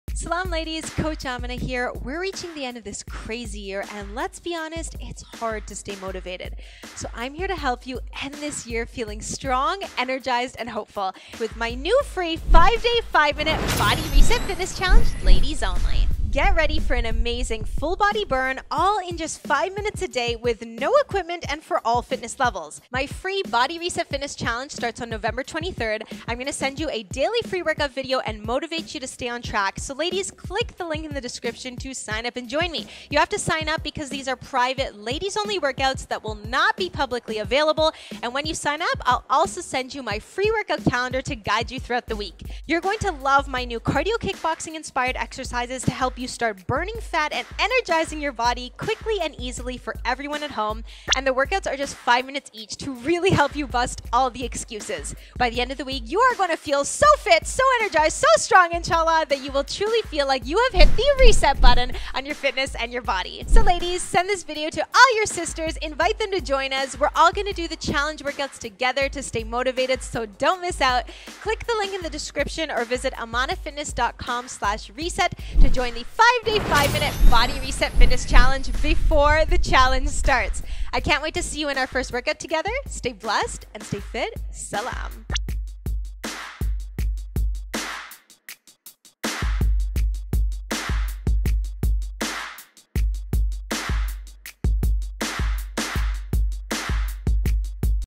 Join my 5-Day BODY⚡️RESET Fitness Challenge! (Ladies-Only!)
This year has been crazy, so this is the motivating BOOST you’ve been waiting for!!
My NEW Free 5-Day Fitness Challenge begins on MONDAY, November 23 - 27th!

5 days of free workouts & incredible motivation! Let’s get back on track together! 🤩

📌 IMPORTANT: This is a FREE private ladies-only challenge. These workouts will NOT be publicly available. Ladies, you have to SIGN UP so I can email you the private links to my free workouts!

If you give me just a few minutes a day... I promise you a HUGE boost in energy, and the kickstart you need to reach your fitness & weight loss goals by the end of the year!

So tell me… Are you IN?? 👇⚡️⚡️💙
Share with your workout buddies!!

Amina Khan @ Amanah Fitness